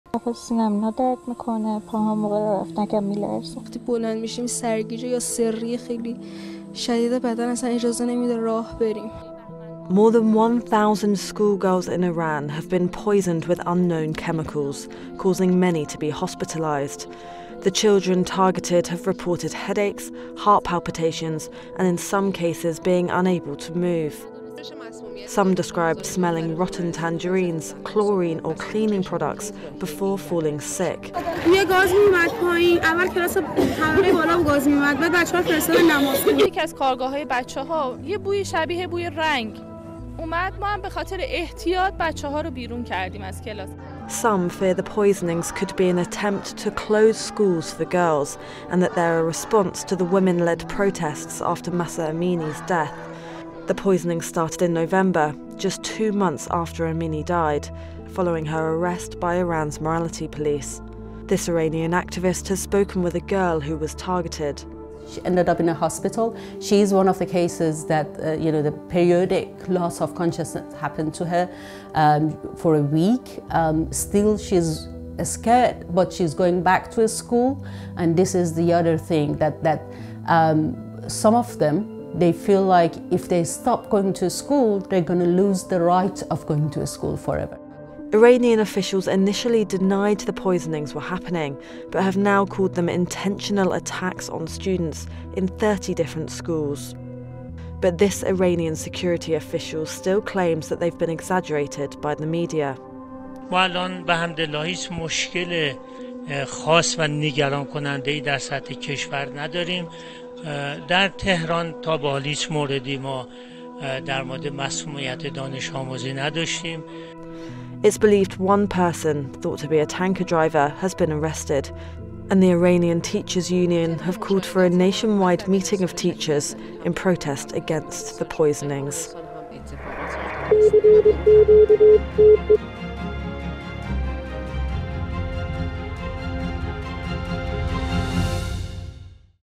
Are Iranian Schoolgirls Being Poisoned?
More than 1000 schoolgirls in Iran have been poisoned with unknown chemicals, causing many to be hospitalised.

The children targeted have reported headaches, heart palpitations, and, in some cases, being unable to move. Some described smelling rotten tangerines, chlorine, or cleaning products before falling sick.

Some fear the poisonings could be an attempt to close schools for girls, and that they’re a response to the women-led protests after Masha Amini’s death.

They started in November, just two months after Amini died, following her arrest by Iran’s morality police.

This Iranian activist has spoken with a girl who has been targeted.

Iranian officials initially denied the poisonings were happening, but have now called them intentional attacks on students in 30 different schools.

It’s believed one person - thought to be a tanker driver - has been arrested, and the Iranian Teachers’ Union have called for a nationwide meeting of teachers, in protest against the poisonings.